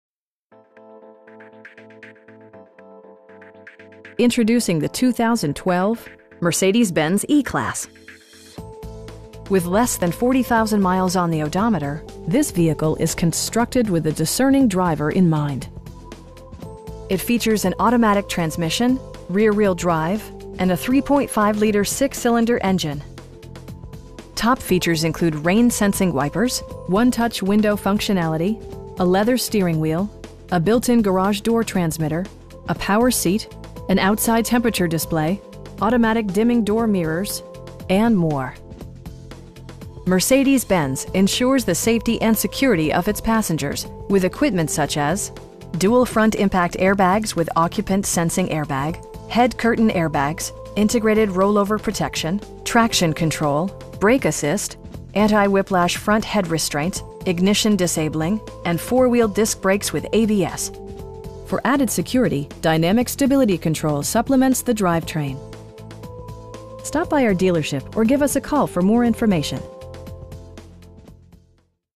2012 Mercedes-Benz E-Class E350 in Dedham, MA 02026
Introducing the 2012 Mercedes-Benz E-Class. With less than 40,000 miles on the odometer, this vehicle is constructed with the discerning driver in mind. It features an automatic transmission, rear-wheel drive, and a 3.5 liter 6 cylinder engine. Top features include rain sensing wipers, 1-touch window functionality, a leather steering wheel, a built-in garage door transmitter, a power seat, an outside temperature display, automatic dimming door mirrors, and more. Mercedes-Benz ensures the safety and security of its passengers with equipment such as: dual front impact airbags with occupant sensing airbag, head curtain airbags, integrated roll-over protection, traction control, brake assist, anti-whiplash front head restraints, ignition disabling, and 4 wheel disc brakes with ABS. For added security, dynamic Stability Control supplements the drivetrain. Our aim is to provide our customers with the best prices and service at all times. Stop by our dealership or give us a call for more information.